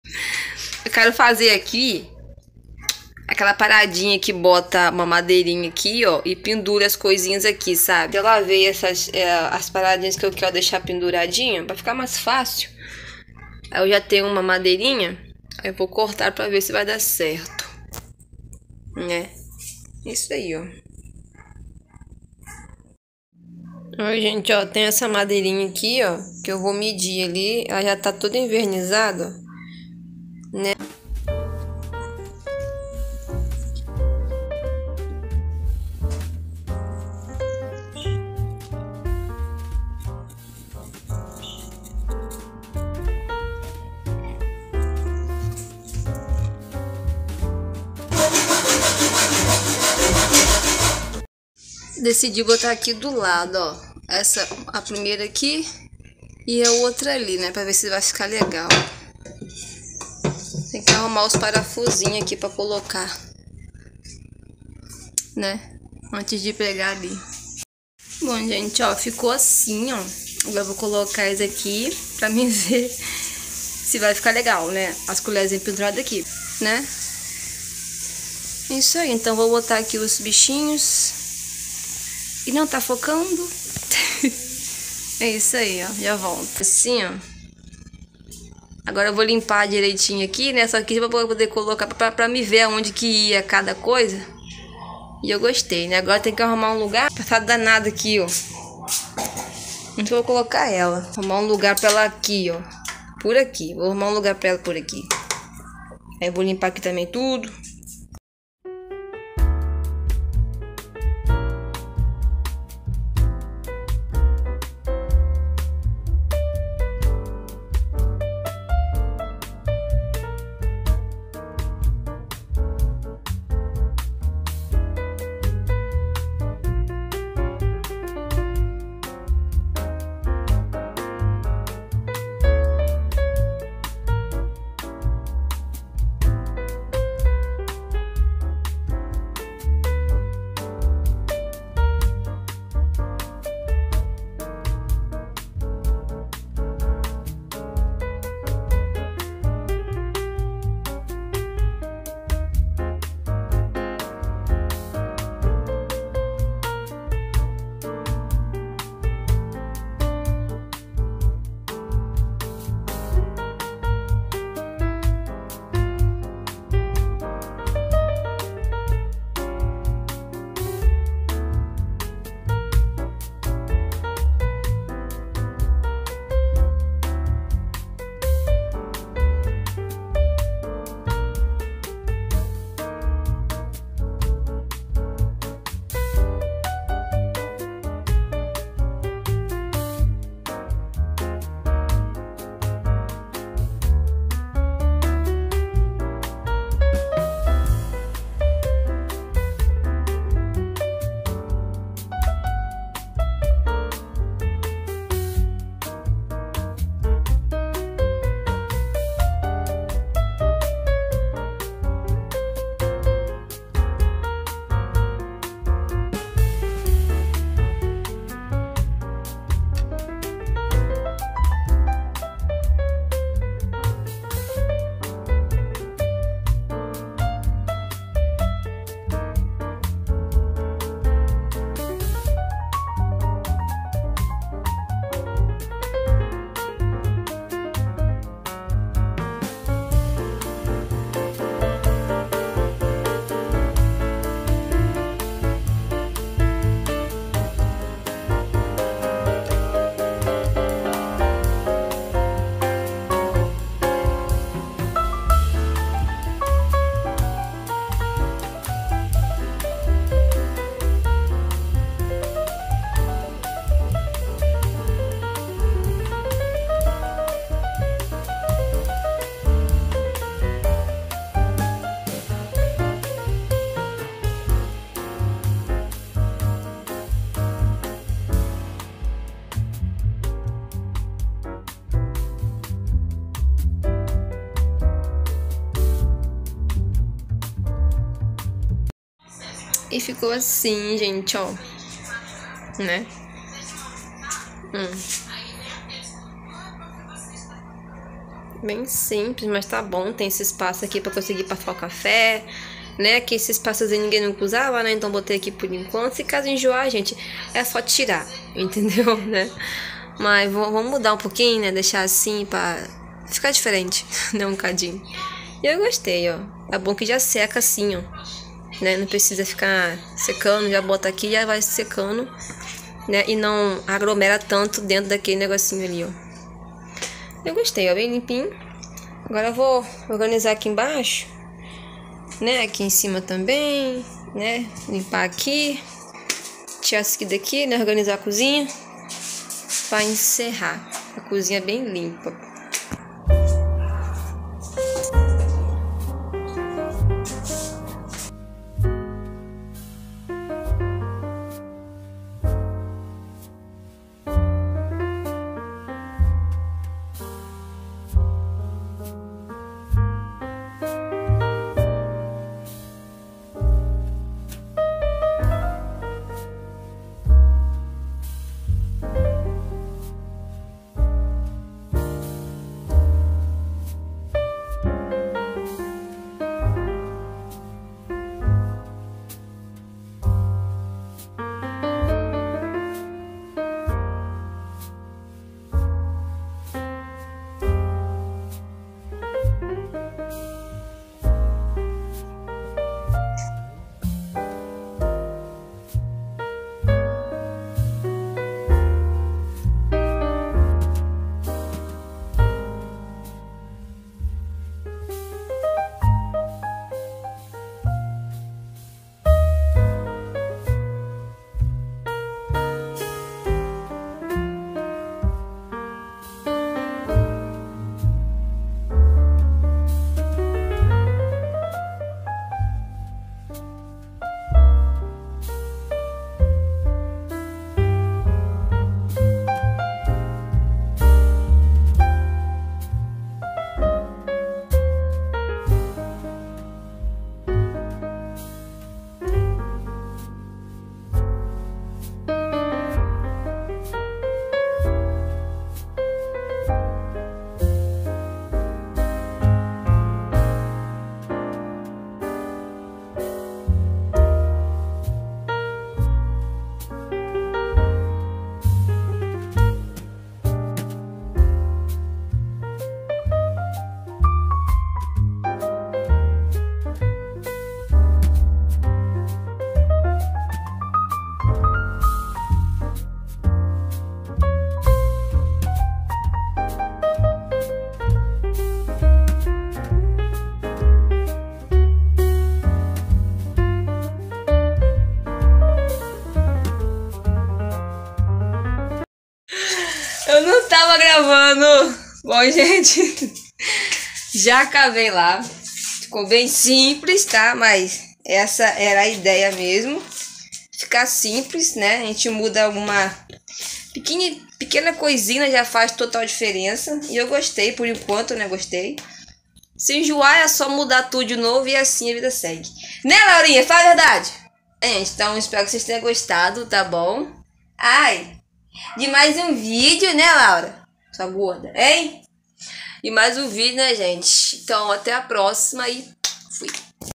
FICOU INCRÍVEL😍 ORGANIZAÇÃO MUDEI A COZINHA - FAXINA NA COZINHA
mudei a minha cozinha poucas coisas faz bastante diferença